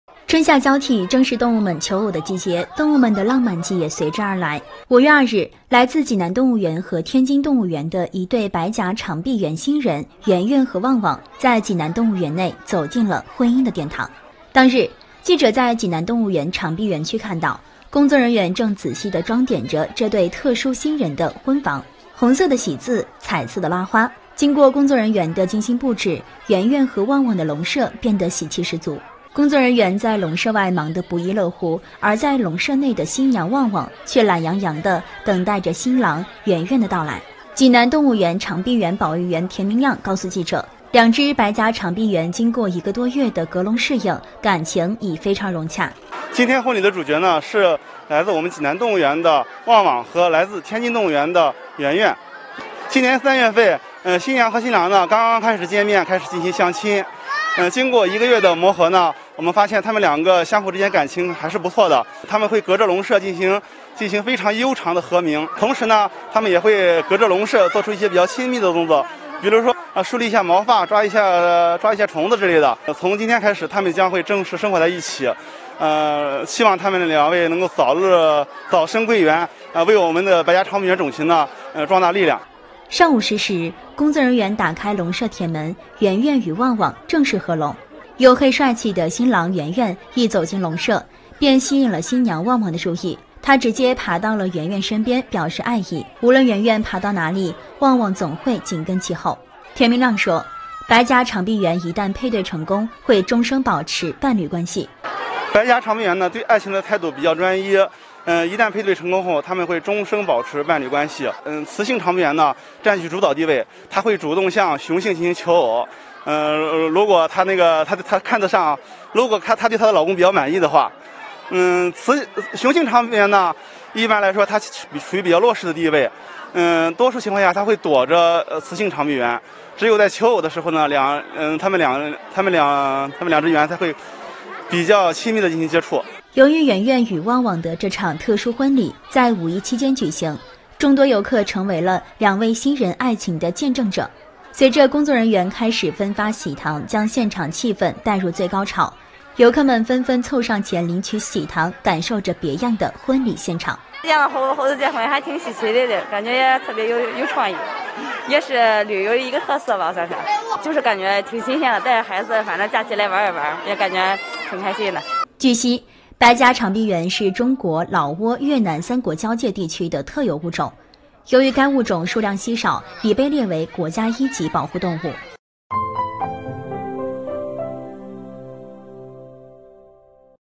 白颊长臂猿举行“婚礼” 动物园发喜糖
春夏交替，正是动物们求偶的季节，动物们的浪漫季也随之而来。5月2日，来自济南动物园和天津动物园的一对白颊长臂猿“新人”圆圆和旺旺在济南动物园内走进了婚姻的殿堂。